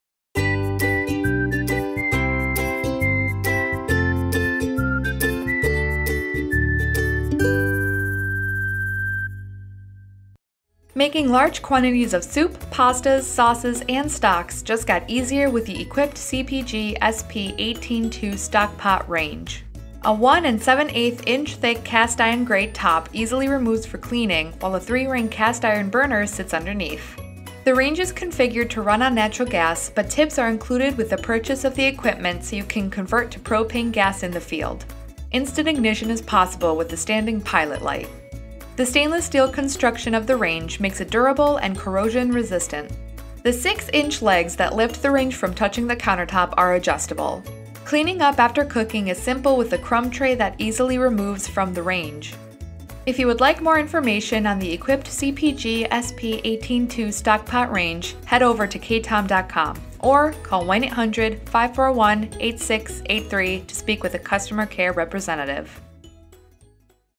eQuipped One-Burner Stock Pot Range
The eQuipped CPG-SP-18-2 stock pot range comes with a heavy-duty, cast-iron grate top that holds your stock pot over a 3-ring cast-iron burner. Two standing pilot lights use natural gas to heat the unit, but tips are included so you can convert it in the field to use propane. The stainless steel construction makes the range durable and easy to keep clean, while its 6-inch adjustable legs lift the unit off of the countertop. A removable crumb tray collects food debris and grease so cleaning up is simple.